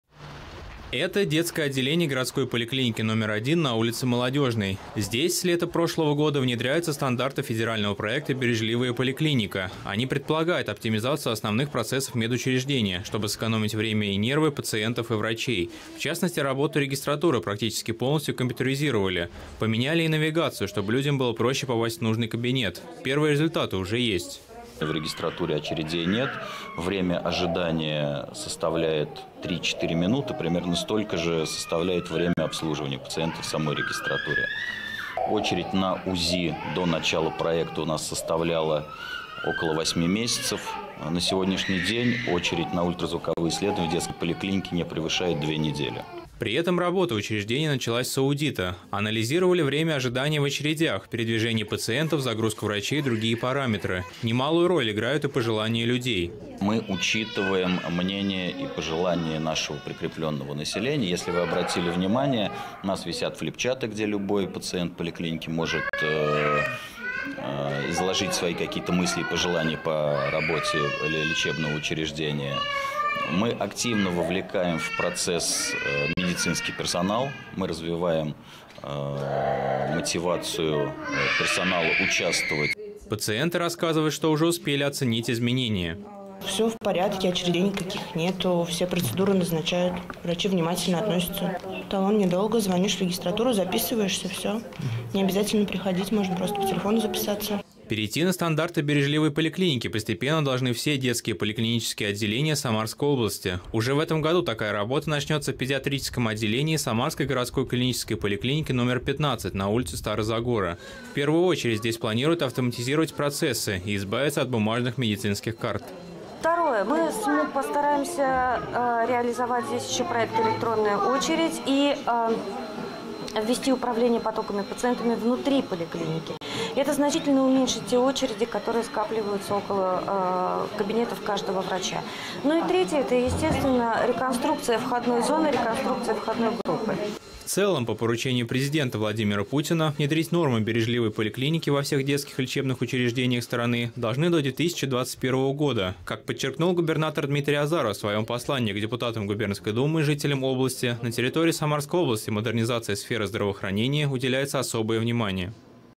Новости Самары. «Бережливая поликлиника»
В Самаре появились первые больницы, где внедрены стандарты федерального проекта «Бережливая поликлиника». Они в разы сокращают очереди, ускоряют сдачу анализов и позволяют обслуживать больше пациентов при том же количестве ресурсов. Конечная цель — сэкономить время врачей и пациентов. Соответствует ли заявленное практике — выяснил наш корреспондент.